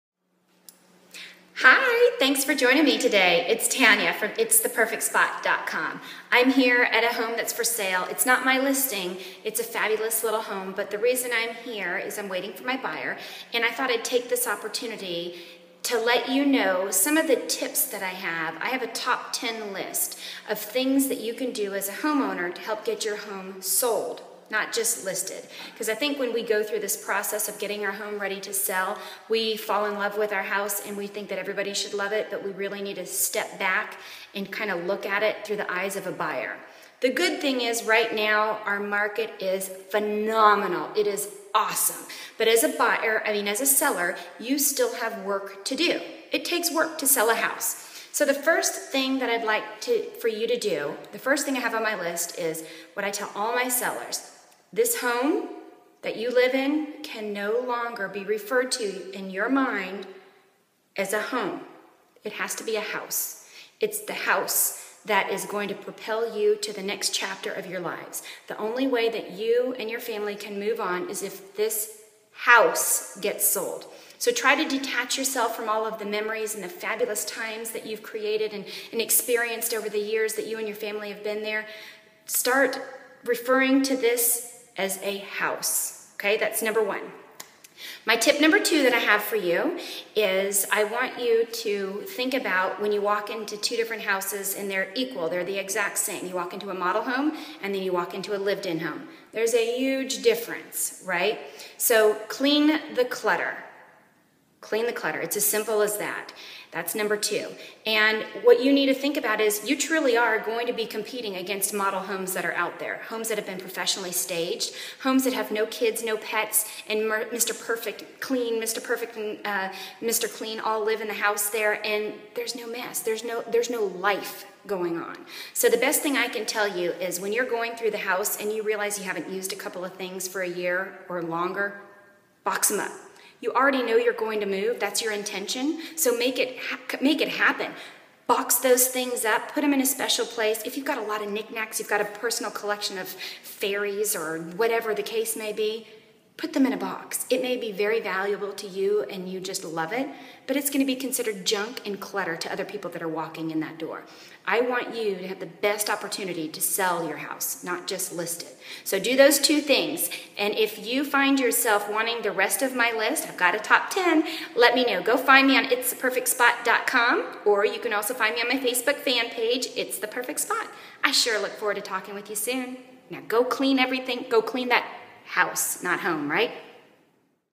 2 Of My Top 10 Strategies To Help SELL Your House
Here are two of my top ten strategies  to help get your house SOLD, not just listed!

#1 start referring to your home as a house. Try to detach yourself from the emotions and memories that the word "home" drums up. This is now a HOUSE that will help propel you and your family to the next stage. It doesn't matter if you are downsizing, buying a larger home or have decided to rent; the point it, if you want to sell it, get serious and make this little adjustment. I promise it will feel easier the more you say it and believe the sale of this house will help you achieve your goals and dreams.

#2 remove the clutter. It's as sole as that! Buyers don't care about your precious crystal fairy collection. To them, it's just clutter and distracting. You already know you are going to move, so let's go ahead and box it up! If you haven't used some things around the house for over a year, that's a good sign it can be donated, boxed up for later use at your new place or even sold! The reality is, your house is not the only one those buyers are going to walk through. So give yours the best possible chance to shine and give those model homes a run for their money! Go "one up" your neighbor house and declutter!